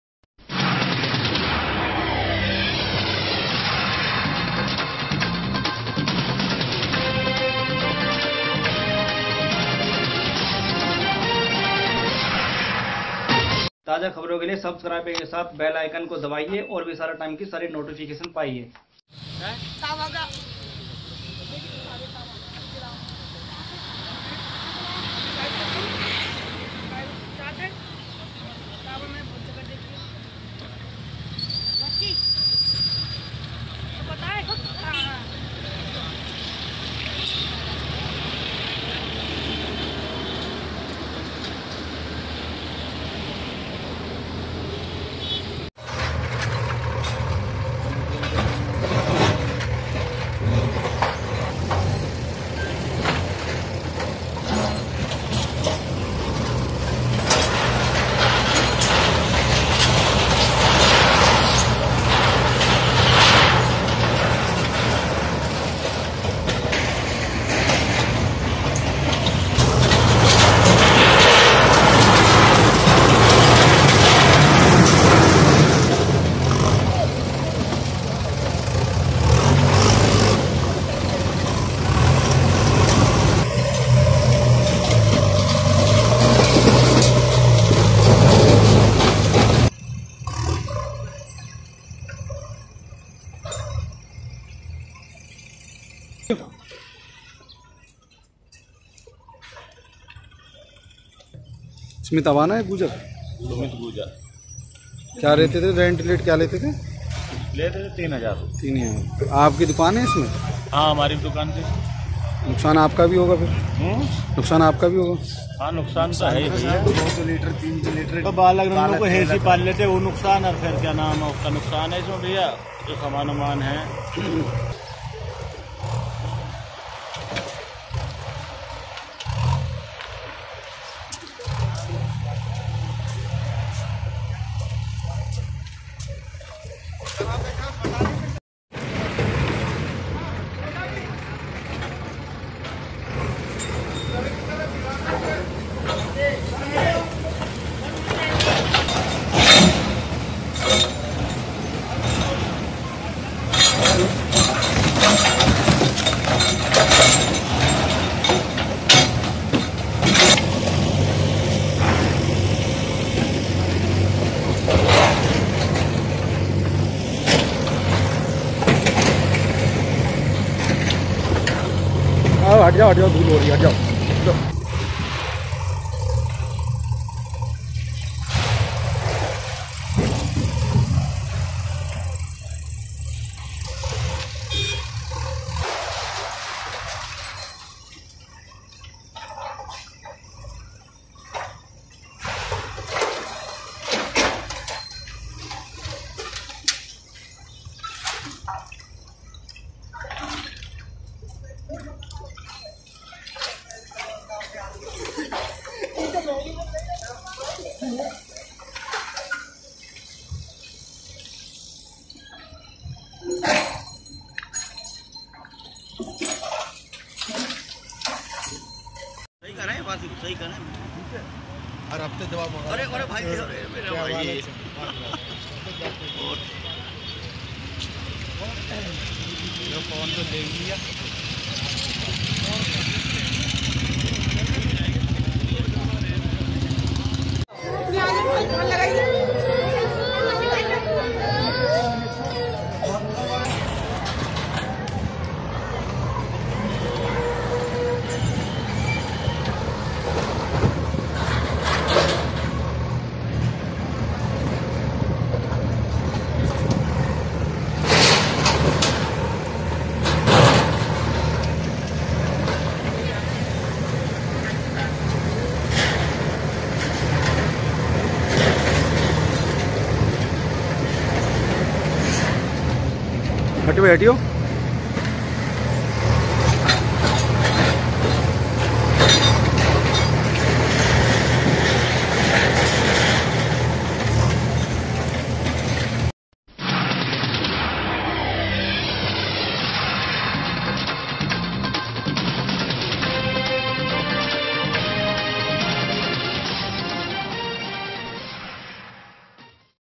नोएडा के बख्तावरपुर गांव में तोड़ी दुकाने"; प्राधिकरण की कार्यवाही
नोएडा के सेक्टर 127 गाँव बख्तावरपुर में नोएडा प्राधिकरण ने की बड़ी कार्यवाही।पुलिस प्रसासन व प्राधिकरण की टीम ने मिलकर साझा कार्यवाही करते हुए जो अवैध आवास व दुकाने बनाई गई थी सबको ध्वस्त कराया गया हालांकि नोएडा प्राधिकरण की टीम को लोगो के विरोध का भी सामना करना पड़ा परंतु विरोध के बावजूद भी टीम ने अपनी सजग कार्यवाही को सम्पूर्ण करते हुए  जमीन को कब्जा मुक्त कराया टीम के पदाधिकारियों ने ये भी बताया कि आगे भी ऐसी कारवाही जारी रहेगी